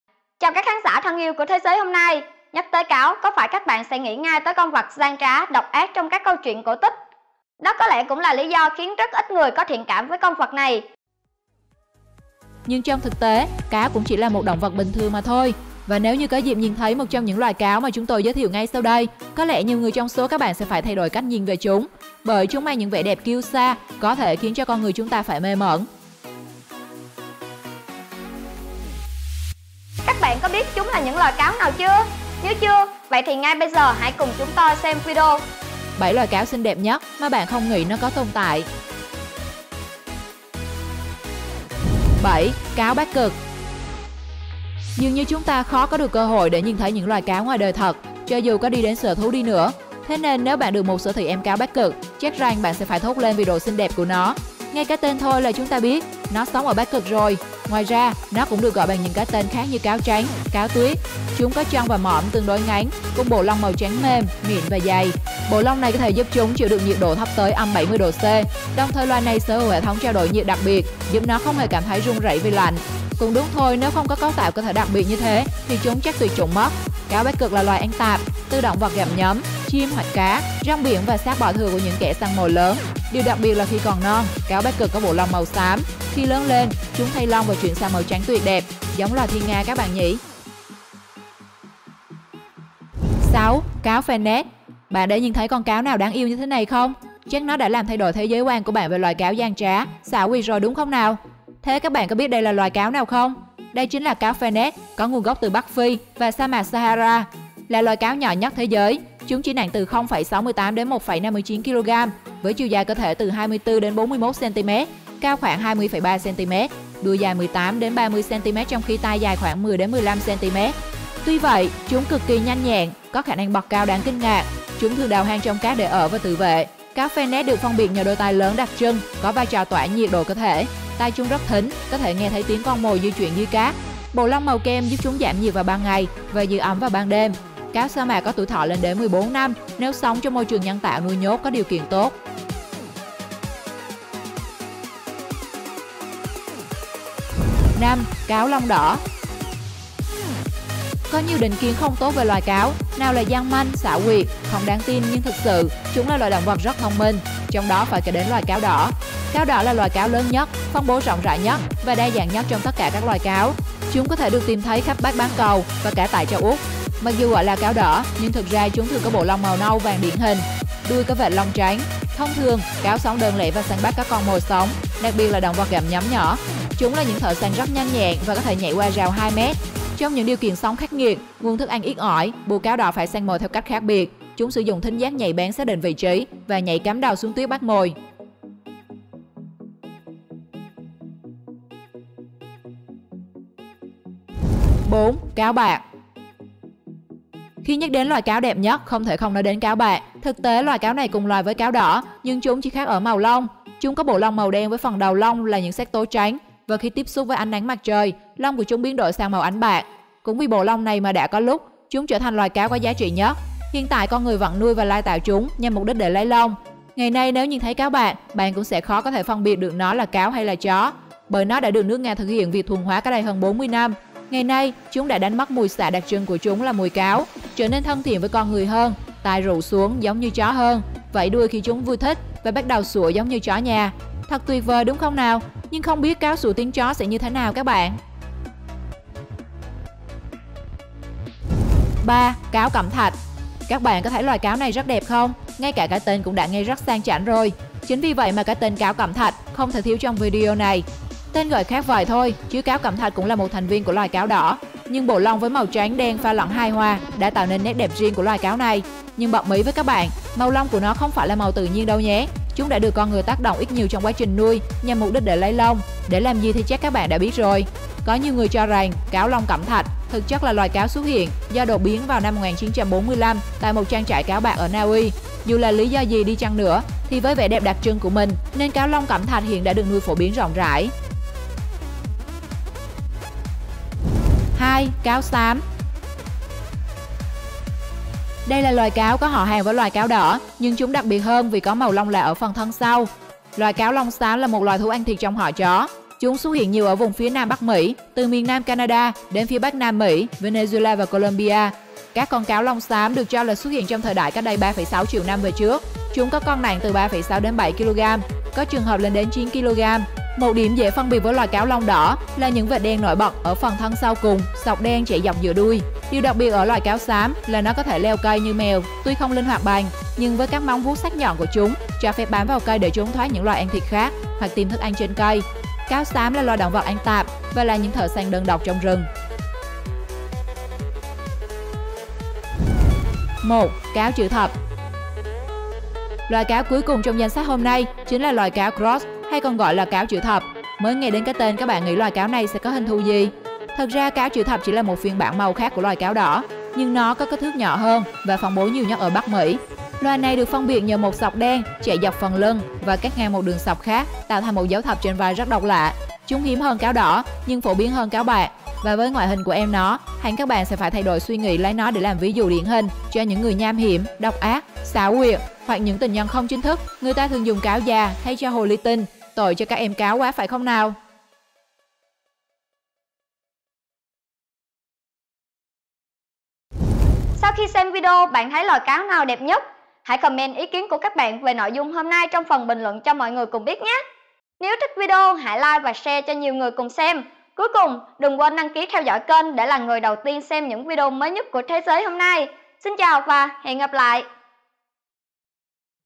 7 Loài Cáo Xinh Đẹp Nhất Mà Bạn Không Nghĩ Nó Còn Tồn Tại
7 Loài Cáo Xinh Đẹp Nhất Mà Bạn Không Nghĩ Nó Còn Tồn Tại

Chào các khán giả thân yêu của Thế giới hôm nay, Nhắc tới cáo, có phải các bạn sẽ nghĩ ngay tới con vật gian trá, độc ác trong các câu truyện cổ tích. Đây có lẽ cũng là lý do khiến rất ít người có thiện cảm với con vật này. Nhưng trong thực tế, cáo cũng chỉ là một động vật bình thường mà thôi, và nếu như có dịp nhìn thấy một trong những loài cáo mà chúng tôi giới thiệu ngay sau đây, có lẻ nhiều người trong số các bạn sẽ phải thay đổi cách nhìn về chúng. Bởi chúng mang những vẻ đẹp kiêu sa có thể khiến cho con người chúng ta phải mê mẩn. Các bạn đã biết chúng là những loài cáo nào chưa? Nếu chưa vậy thì ngay bây giờ hãy cùng chúng tôi xem video 7 Loài Cáo Xinh Đẹp Nhất Mà Bạn Không Nghĩ Nó Có Tồn Tại.

7. Cáo Bắc cực
6. Cáo Fennec
5. Cáo lông đỏ
4. Cáo bạc
3. Cáo cẩm thạch
2. Cáo xám
1. Cáo chữ thập